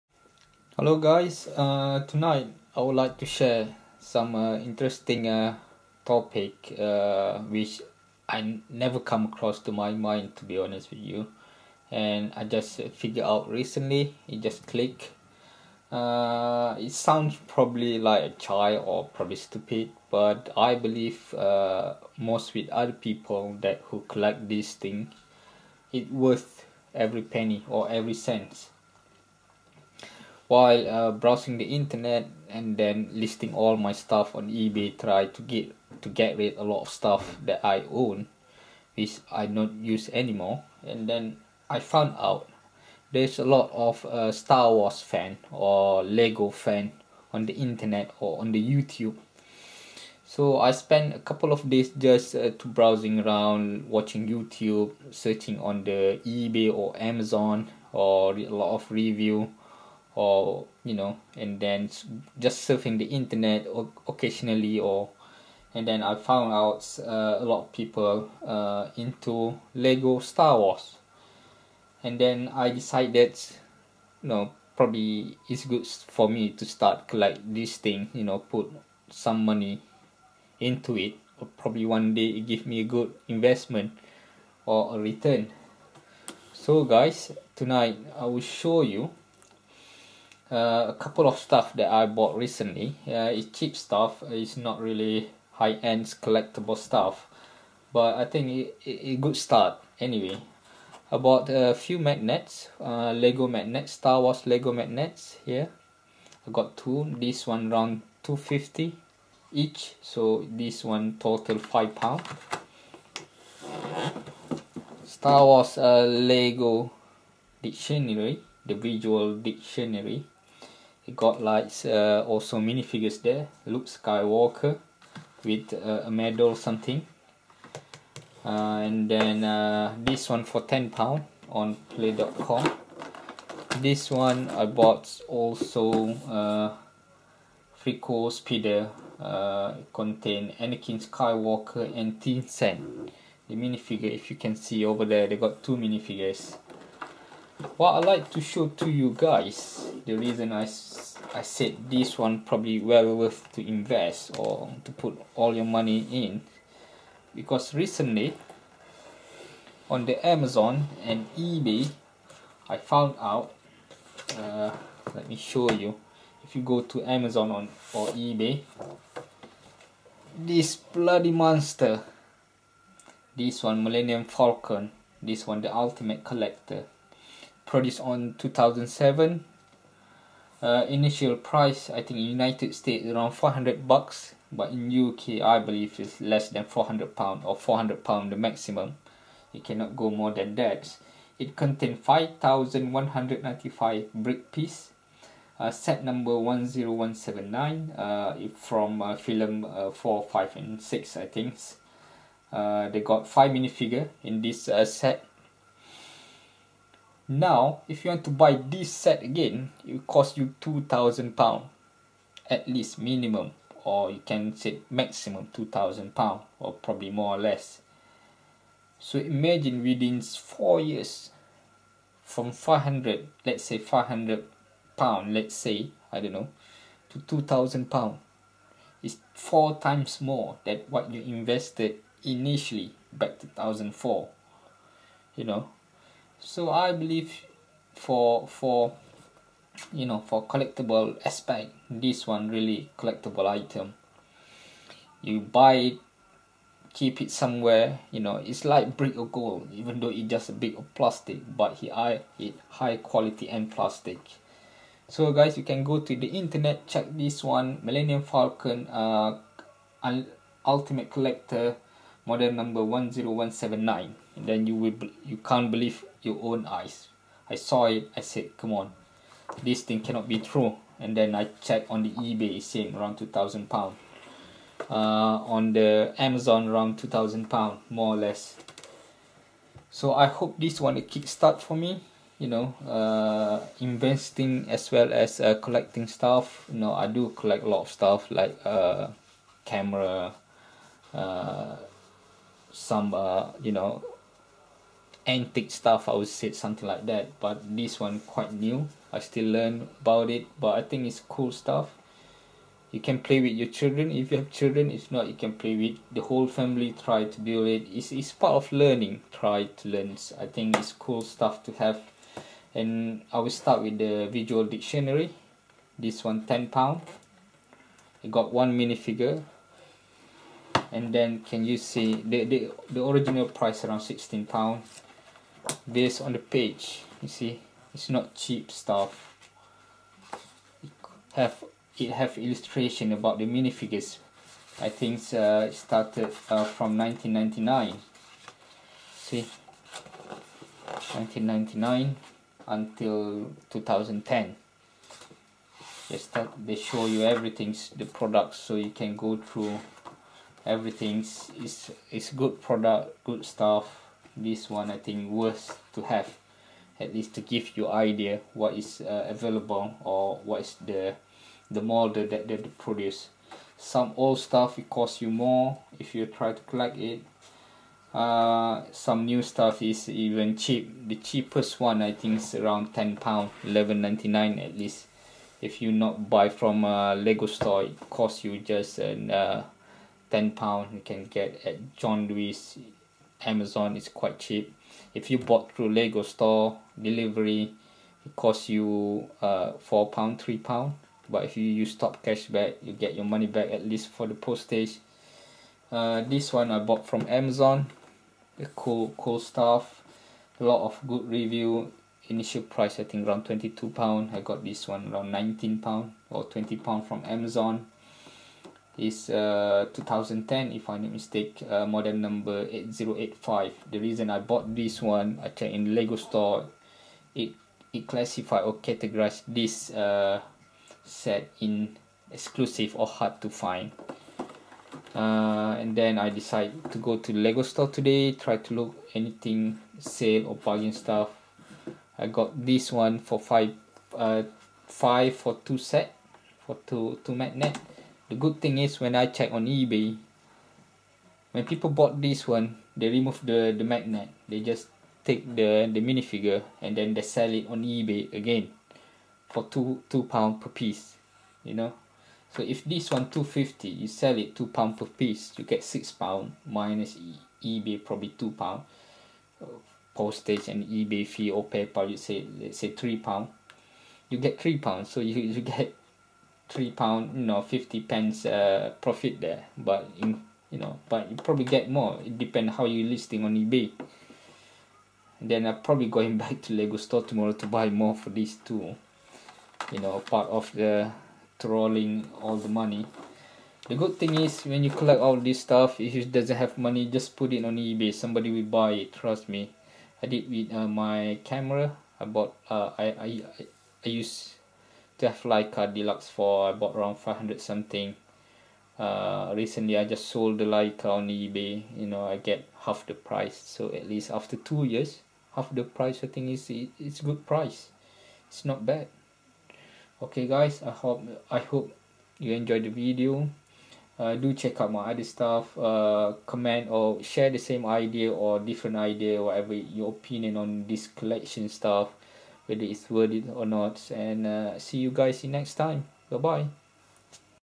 Lego what do u think? hobby, toy or good investment?
As I did mentioned in the video, lego is a cool stuff to collect as it hold it value. Please check this amazon link for proof: